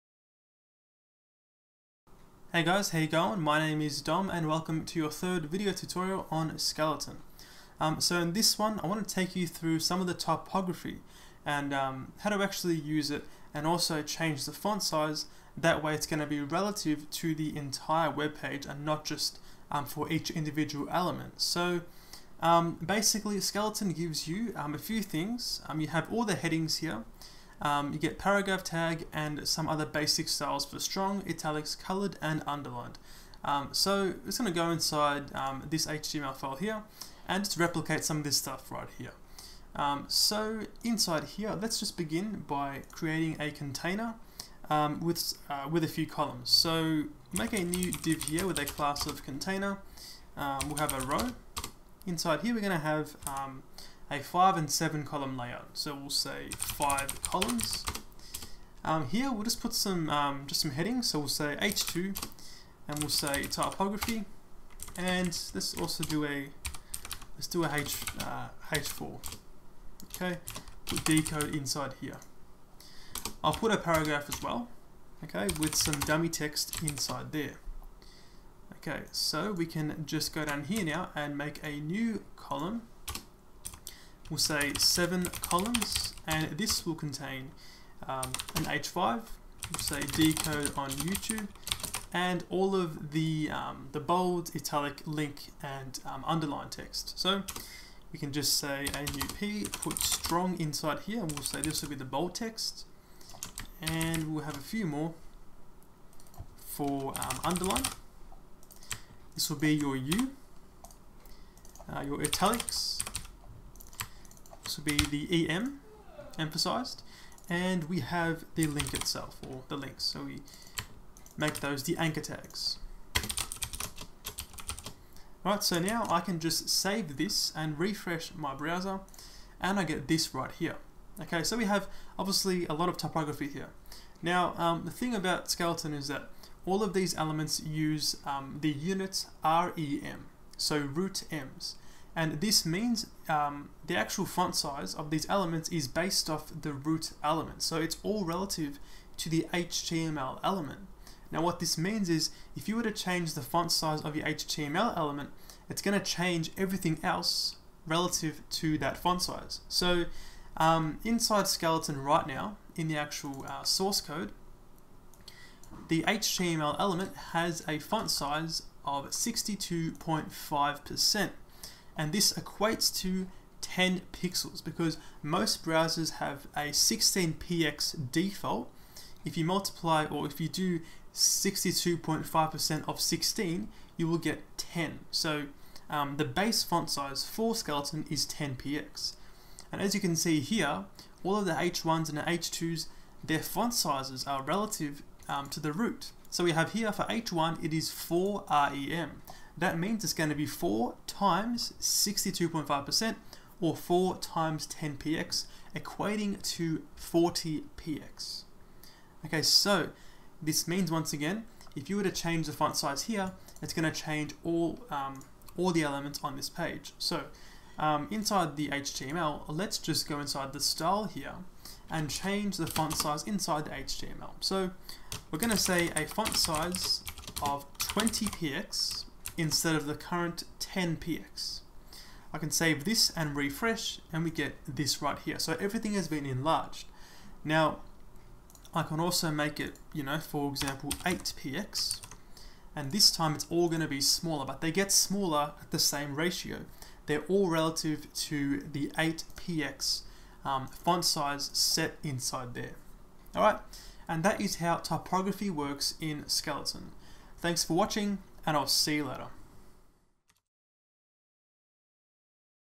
Skeleton CSS Boilerplate Tutorial #3: Typography & Fonts
Skeleton is a responsive CSS Boilerplate that gets your websites or web applications up and running. It's a great starting point for any small-to-mid size web application.

In this video I take you through how the typography and fonts work within Skeleton, and how it takes advantage of the rem CSS unit to create consistent font sizes based on the root element (HTML).